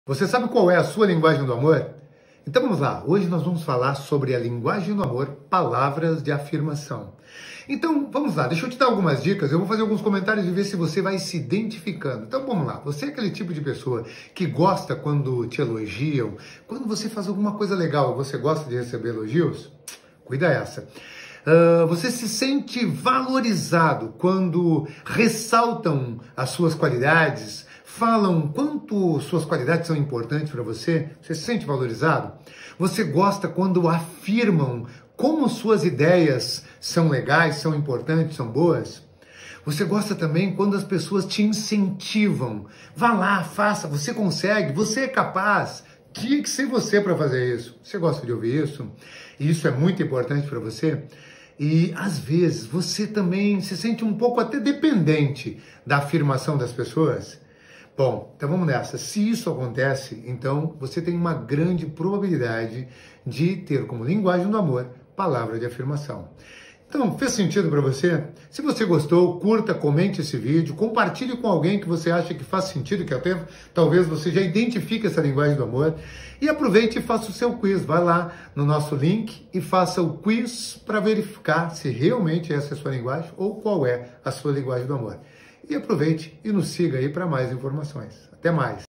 shortsvideo shorts coaching linguagensdoamor
Nossa, como você ficou bem com essa roupa!
Que ótima ideia, você é muito criativo!
Eu já disse hoje o quanto você é especial?

Muitas vezes você até já sabe disso tudo, mas é tão bom ouvir, né?
Especialmente se a sua Linguagem de Amor é PALAVRAS DE AFIRMAÇÃO 💬

Assiste o vídeo até o final e descubra se você ou o seu amor são abastecidos dessa forma